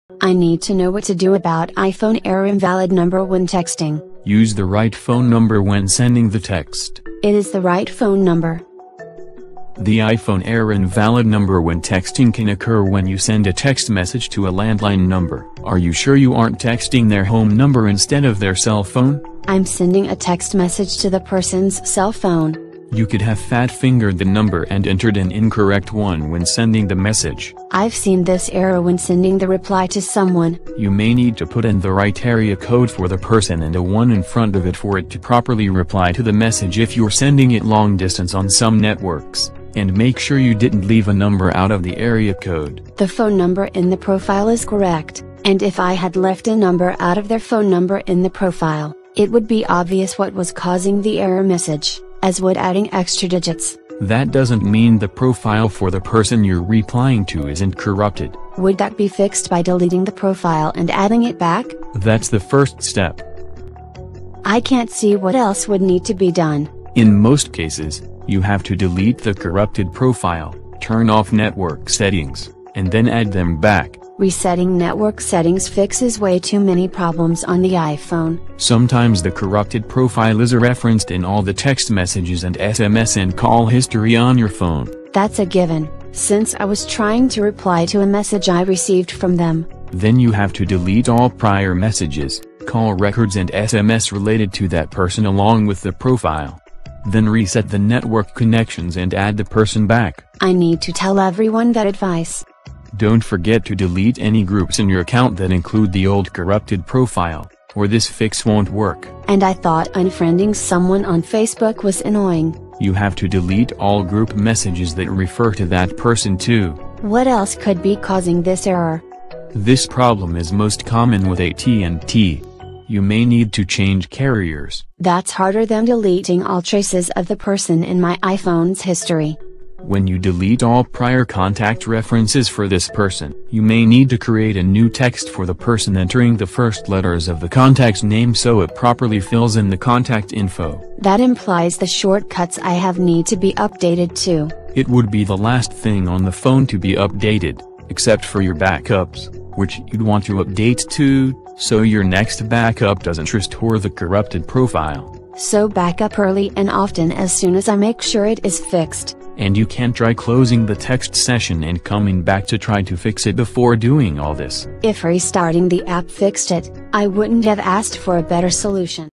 iPhone Error Invalid Number When Texting
I need to know what to do about iPhone error invalid number when texting.

Use the right phone number when sending the text.

It is the right phone number.

The iPhone error Invalid Number When Texting can occur when you send a text message to a landline number. Are you sure you aren’t texting their home number instead of their cell phone?

I’m sending a text message to the person’s cell phone.

You could have fat-fingered the number and entered an incorrect one when sending the message.

I’ve seen this error when sending the reply to someone.

You may need to put in the right area code for the person and a one in front of it for it to properly reply to the message if you’re sending it long distance on some networks. And make sure you didn’t leave a number out of the area code.

The phone number in the profile is correct. And if I had left a number out of their phone number in the profile, it would be obvious what was causing the error message, as would adding extra digits.

That doesn’t mean the profile for the person you’re replying to isn’t corrupted.

Would that be fixed by deleting the profile and adding it back?

That’s the first step.

I can’t see what else would need to be done.

In most cases, you have to delete the corrupted profile, turn off network settings, and then add them back.

Resetting network settings fixes way too many problems on the iPhone.

Sometimes the corrupted profile is referenced in all the text messages and SMS and call history on your phone.

That’s a given, since I was trying to reply to a message I received from them.

Then you have to delete all prior messages, call records and SMS related to that person along with the profile. Then reset the network connections and add the person back.

I need to tell everyone that advice.

Don’t forget to delete any groups in your account that include the old corrupted profile, or this fix won’t work.

And I thought unfriending someone on Facebook was annoying.

You have to delete all group messages that refer to that person too.

What else could be causing this error?

This problem is most common with AT&T. You may need to change carriers.

That’s harder than deleting all traces of the person in my iPhone’s history.

When you delete all prior contact references for this person, you may need to create a new text for the person entering the first letters of the contact’s name so it properly fills in the contact info.

That implies the short cuts I have need to be updated too.

It would be the last thing on the phone to be updated, except for your backups, which you’d want to update too, so your next backup doesn’t restore the corrupted profile.

So back up early and often as soon as I make sure it is fixed.

And you can try closing the text session and coming back to try to fix it before doing all this.

If restarting the app fixed it, I wouldn’t have asked for a better solution.